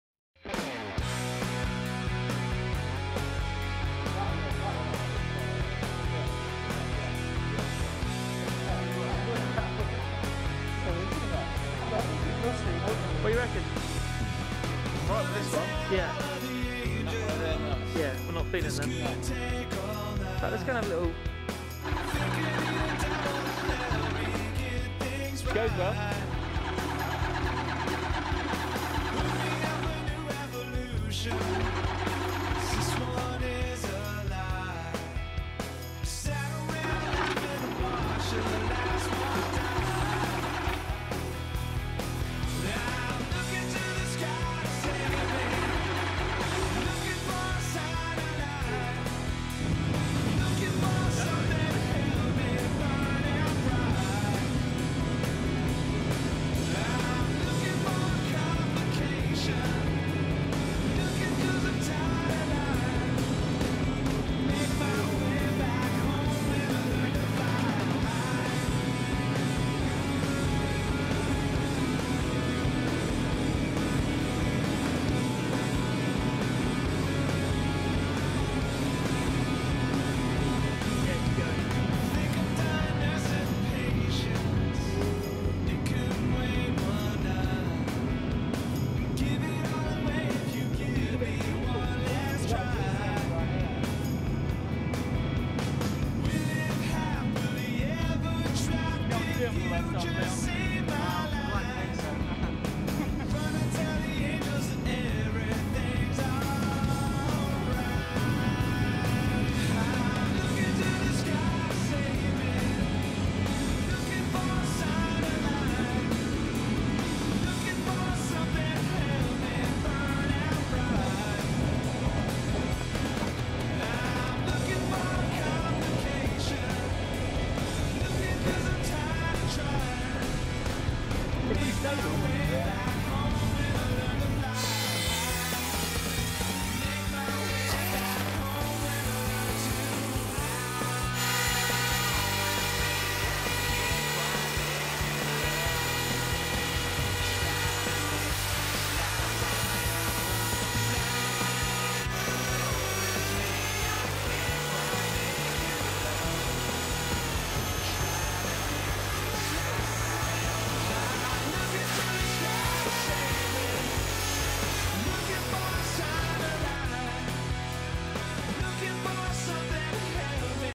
Leon Camier and Leon Haslam flying before Donington Race 2011
Pre Donington Fun